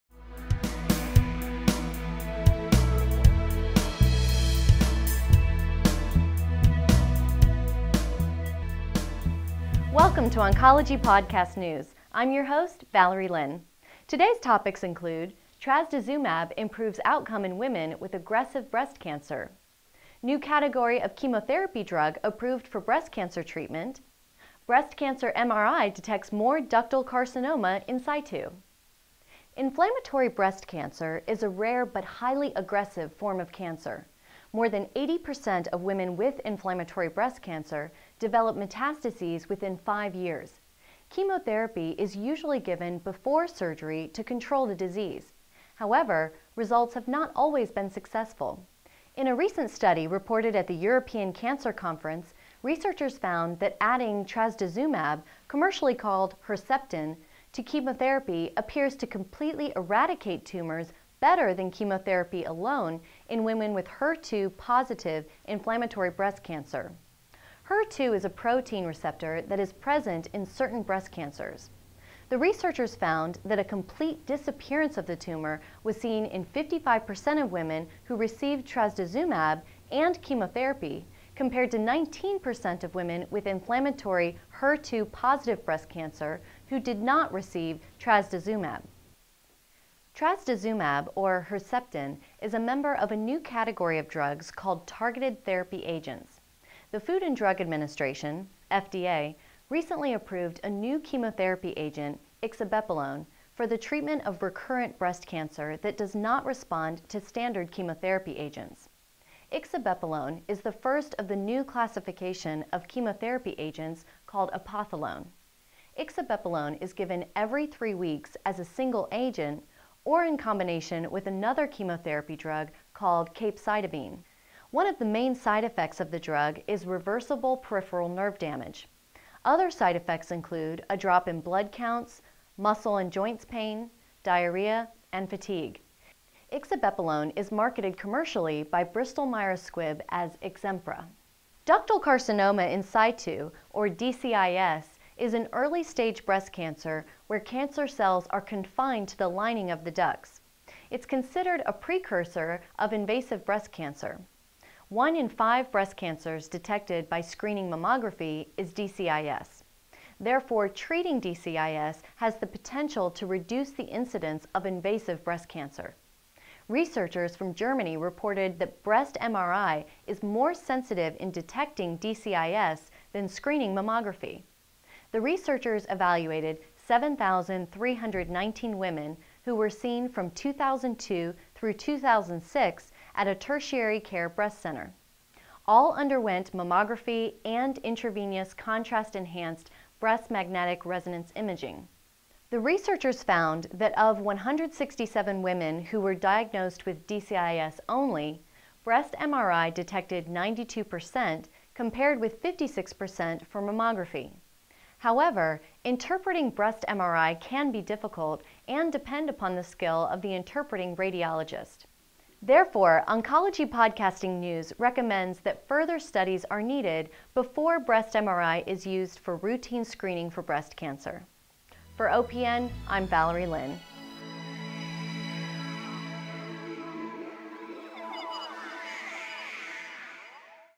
New type of chemotherapy drug approves for breast cancer
Today's topics include: Trastuzumab improves outcome in women with aggressive breast cancer, New type of chemotherapy drug approves for breast cancer treatment, Breast MRI detects more  Ductal carcinoma in situ